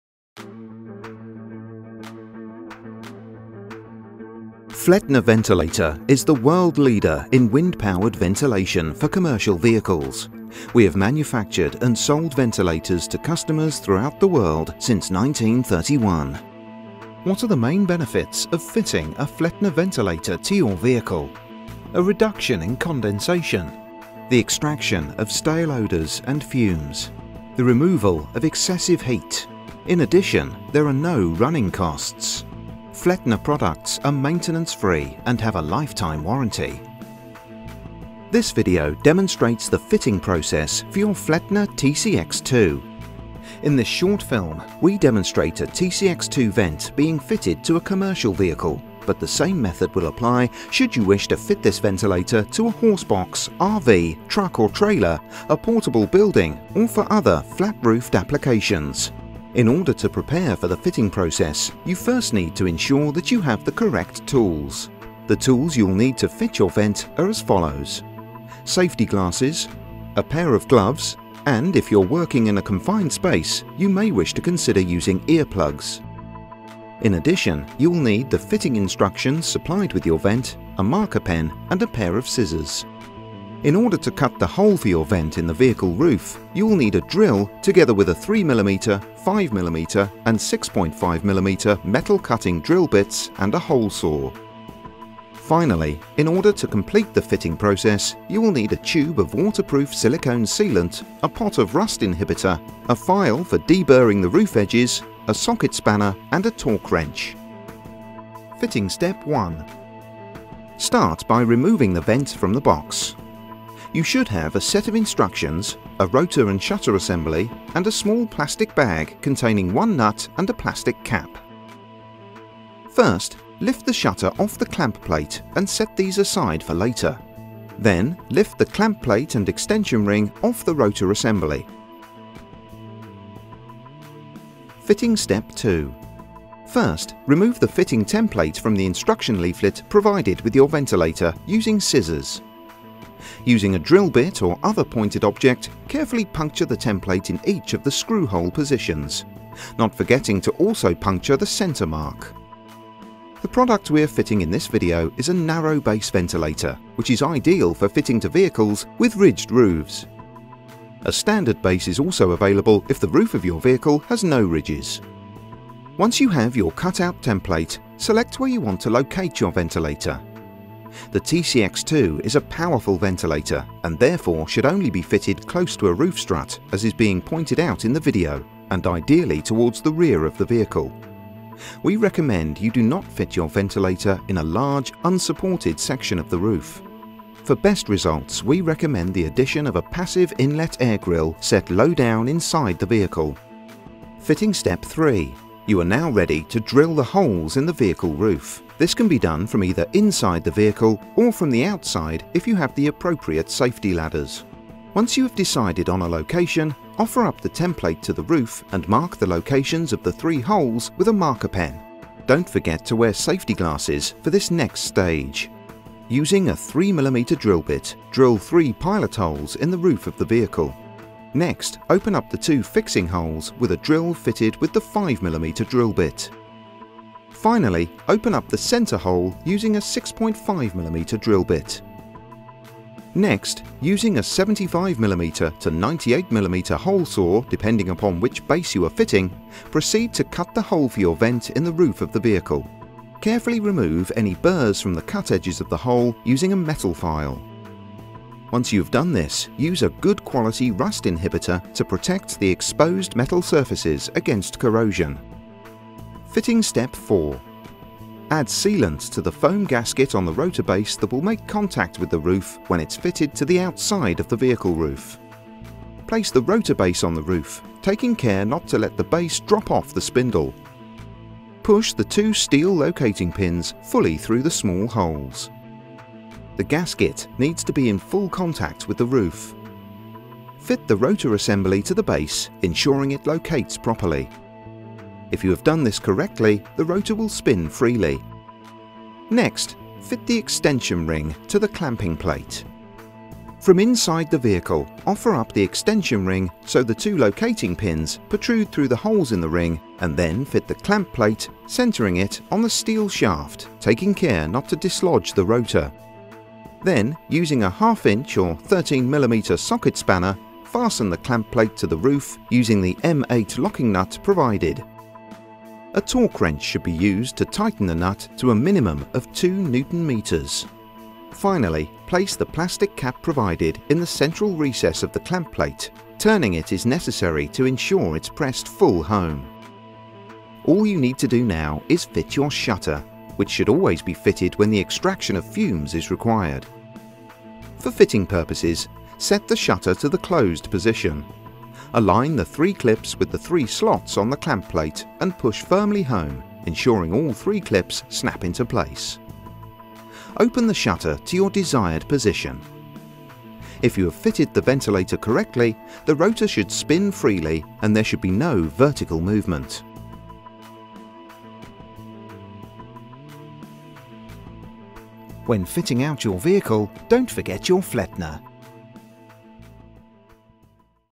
Zo bouw je een Flettner TCX-2 mechanische dakventilator in
Flettner dakventilatoren zijn door de wind aangedreven en zorgen voor een gezonde toe- en afvoer van lucht. Ze zijn eenvoudig te monteren en hebben een goede prijs-kwaliteit verhouding.
De Flettner TCX-2TM is een nieuwe roterende ventilator die twee keer zoveel lucht afvoert als de Flettner 2000.